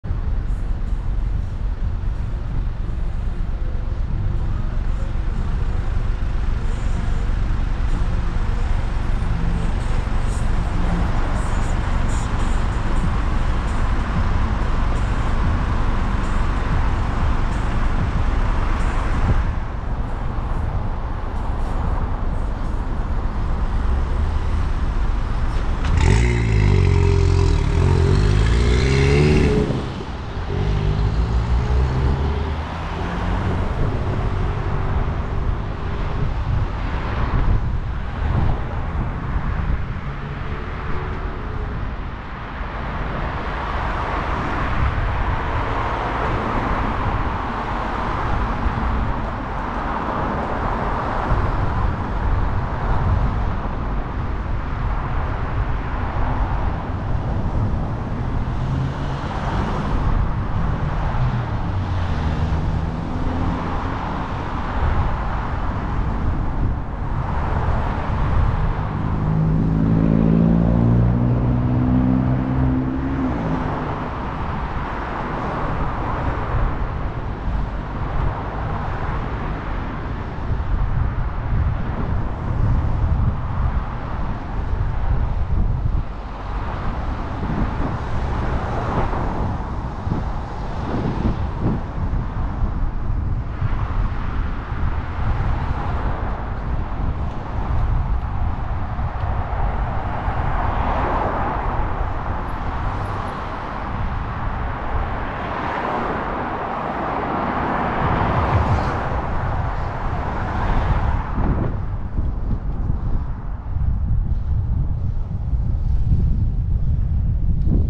I wonder why few people will even contemplate cycling through Astley Bridge.
Could it be that they haven't been encouraged enough?